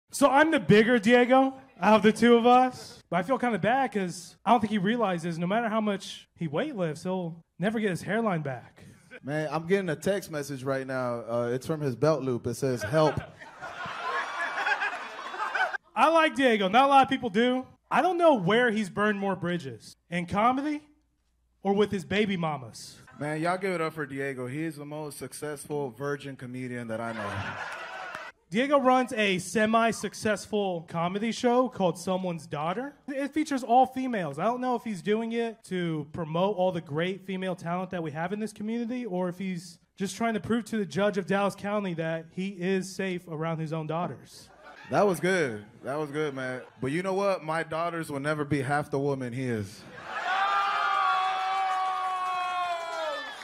Saturday Night Scorchers Roast Battle Short - Round 2 - Diego Morales vs Diego Rangel
Diego Morales Bio:

Diego "Texas Chuckle" Morales is a stand up comedian, radio host, actor, producer, bad softball player and social media influencer based in Texas.

Voted Best of Dallas- Social Media Influencer, Diego takes comedy to an interactive level with his personal connection to his audience.

In addition to his "work", Diego is also the proud (non child support paying) father of four girls, who also works to promote females in comedy.

Diego Rangel Bio:

Diego Rangel is a young comedian whose bittersweet personality charms everyone but his mother. Although being a young buck comes with little life experience, that doesn’t stop him from taking the stage and sharing his bizarre view on the world though jokes that teeter the line between socially acceptable and downright outrageous!